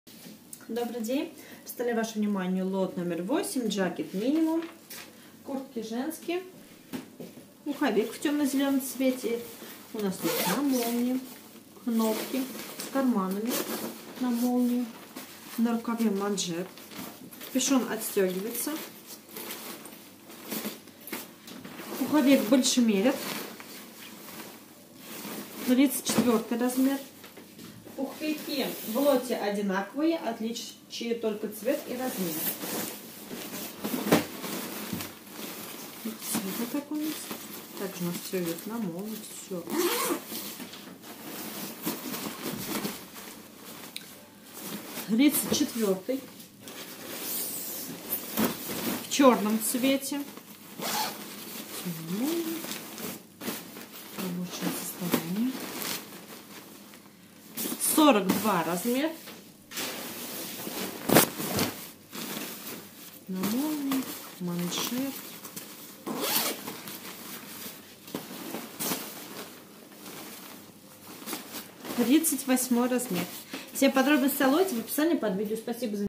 Minimum Jackets 8,сток одежда оптом
Minimum Jackets (лот №8)
Куртки женские зима  (сток)
Страна: Дания
Сезон: Зима
Цена за ед.: 68.25 евро
Кол-во в мешке,шт: 4
Цена за мешок: 273.0 евро